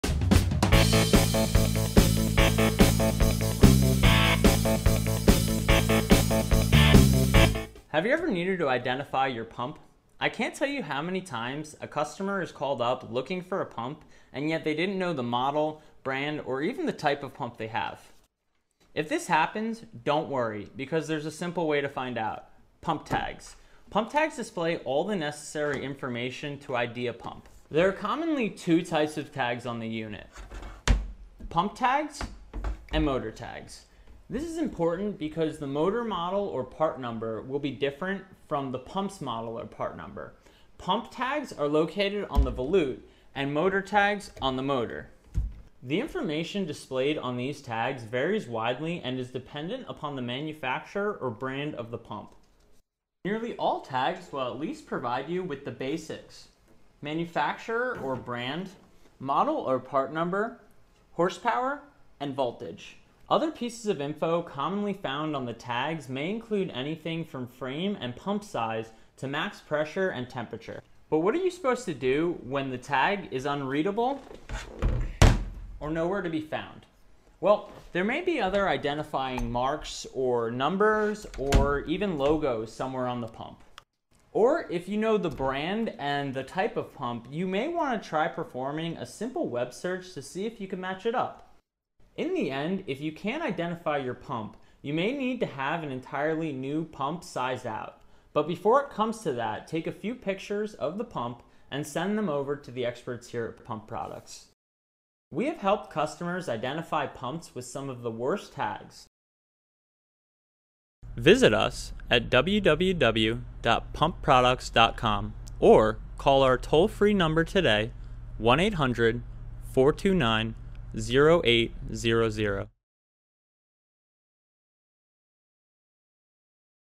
How To Use Pump Tags to Identify Your Pump
This video demonstrates how to use pump tags to gather the necessary information to find out what type of pump you have. Pump tags will give you basic information such as the manufacturer, model number, horsepower and voltage. This information is absolutely critical if you need to replace a damaged pump. If the pump tag is degraded or illegible, try calling the experts at Pump Products at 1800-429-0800 for assistance.